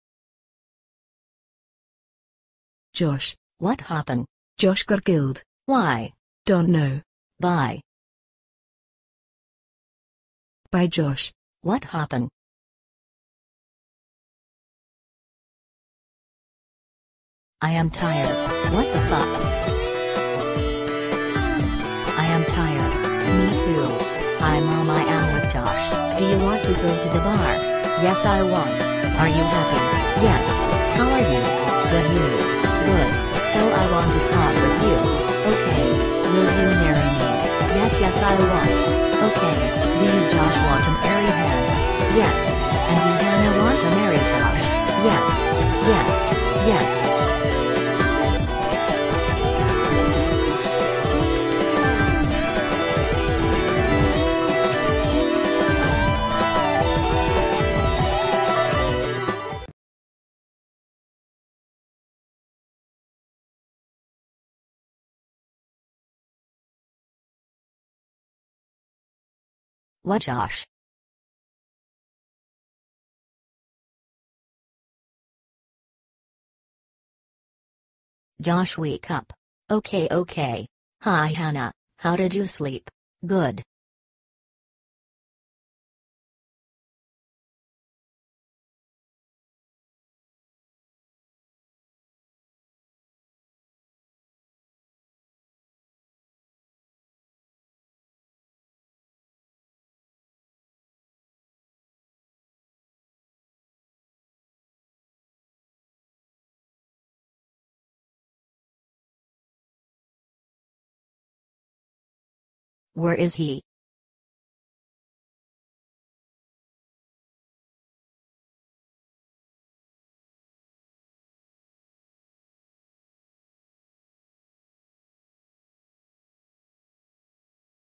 josh and the love 2
josh mery hanna and he want to kill a guy